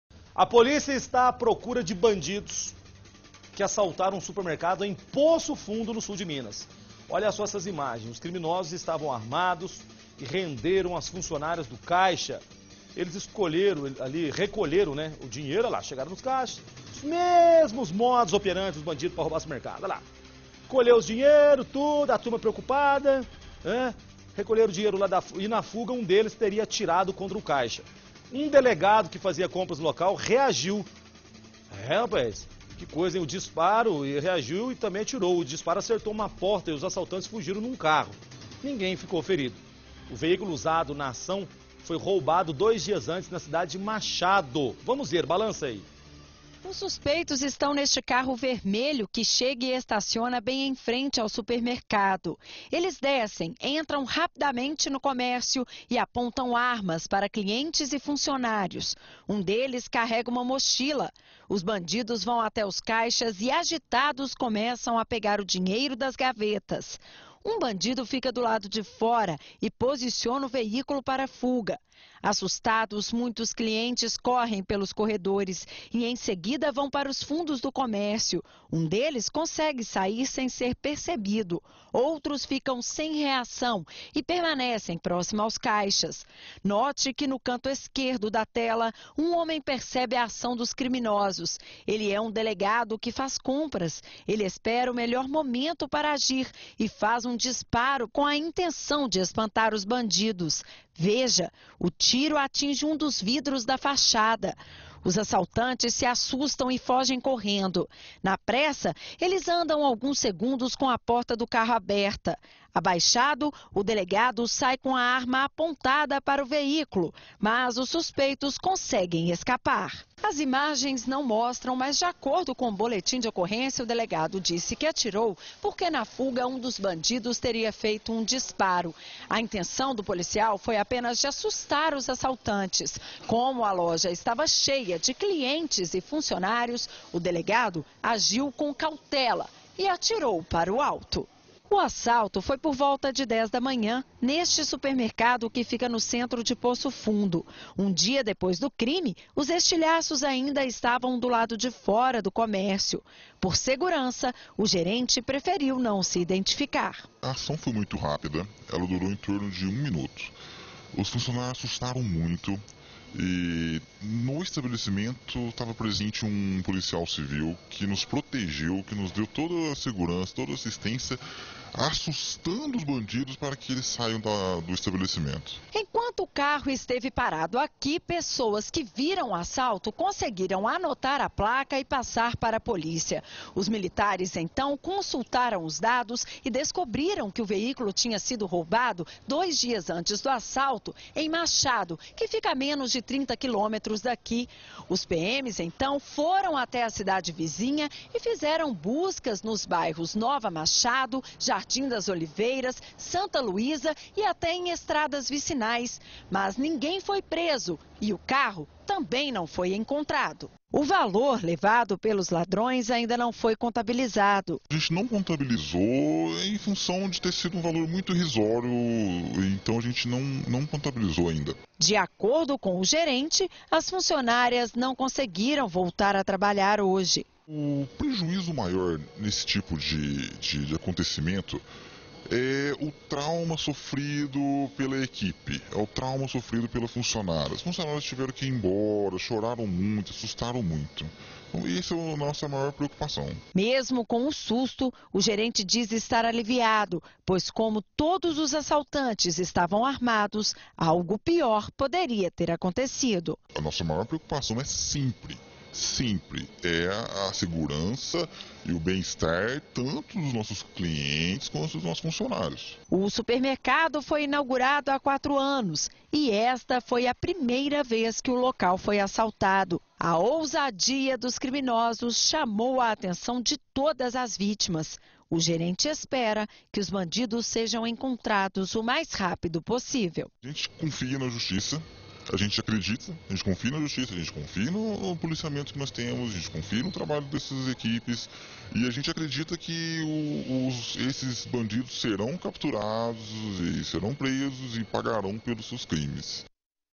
Violência: bandidos assaltam supermercado e fogem em carro roubado
A polícia do Sul de Minas procura por bandidos que assaltaram um supermercado de Poço Fundo. O circuito de segurança registrou o roubo. Os criminosos estavam armados e renderam as funcionárias do caixa. Eles recolheram o dinheiro e na fuga, um deles teria atirado pra dentro da loja. Um delegado que fazia compras no local reagiu ao disparo e atirou contra os criminosos. O tiro acertou uma porta de vidro do supermercado. Os assaltantes fugiram em um carro. Os militares descobriram que o veículo usado foi roubado dois dias antes na cidade de Machado, que fica a menos de trinta quilômetros de Poço Fundo.